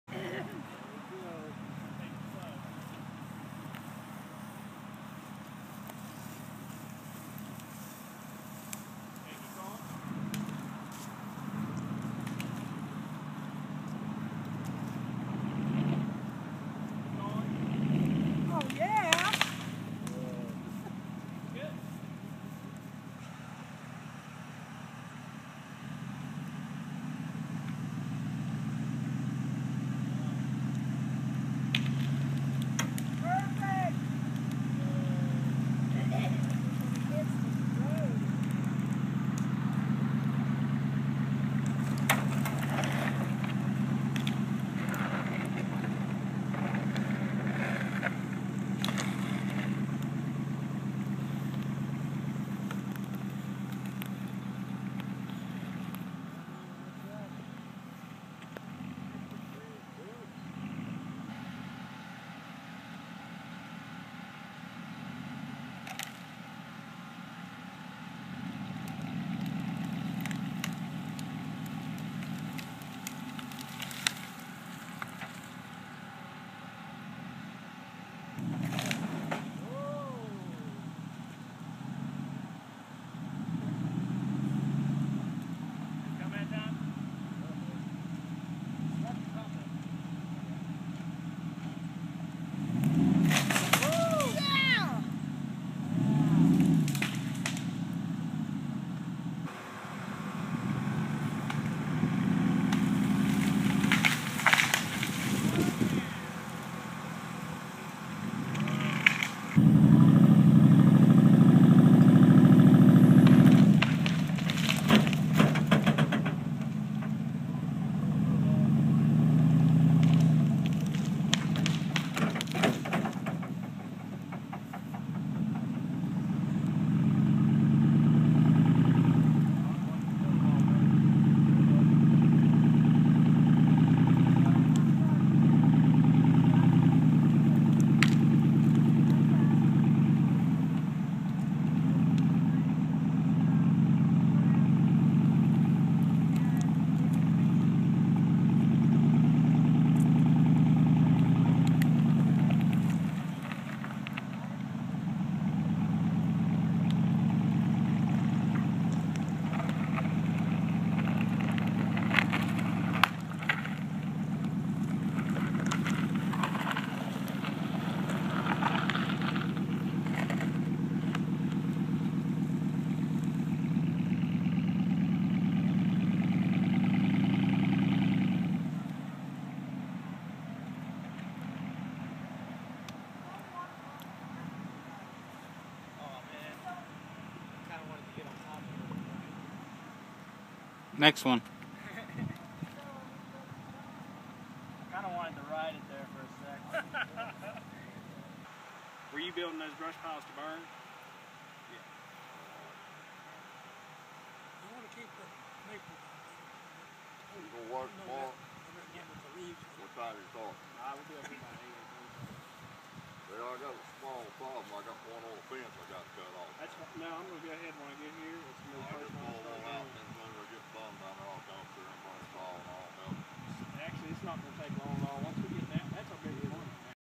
Coral Ridge Monster Truck Tree Jam
Cousin Adam hauling away the fallen trees from The Wind Storm of 2012.